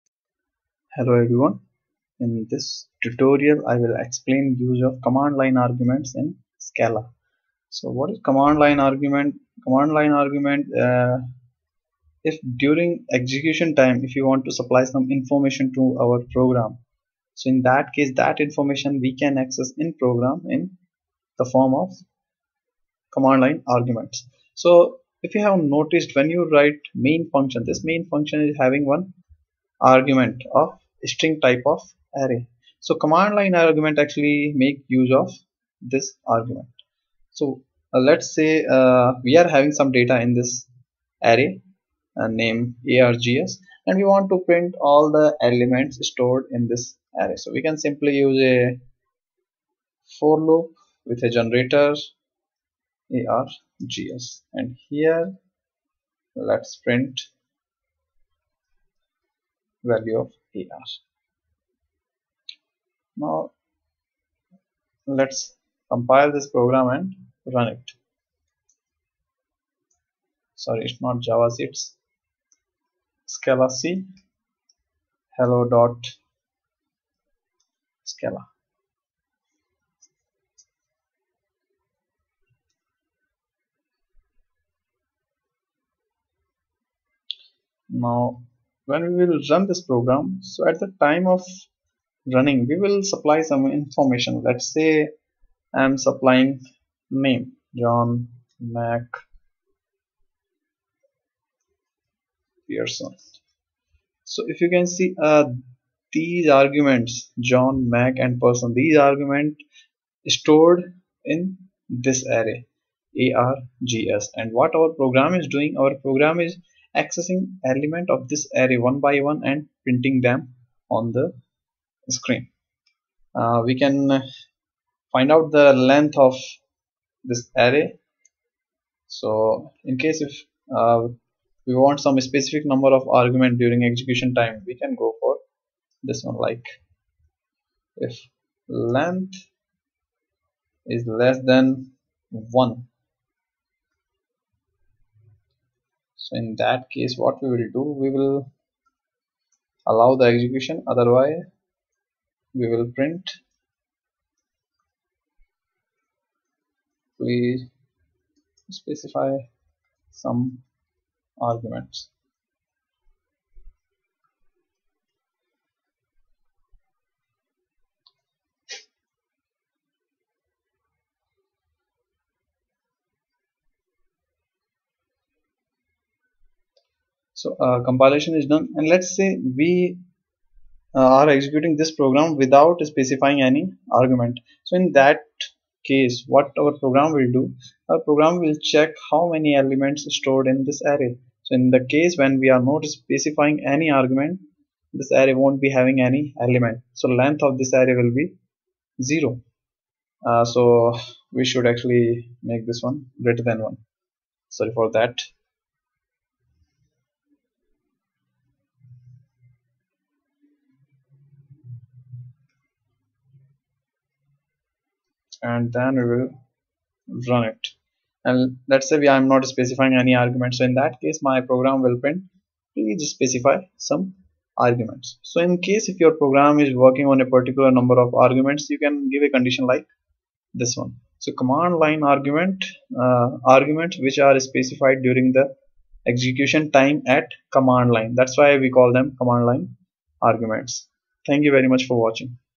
What is Command Line Arguments in Scala # Scala Tutorial -11
Command line arguments to a Scala script are available via a Scala array named args . In Scala, arrays are zero based.
This video will teach you how to use command line arguments in scala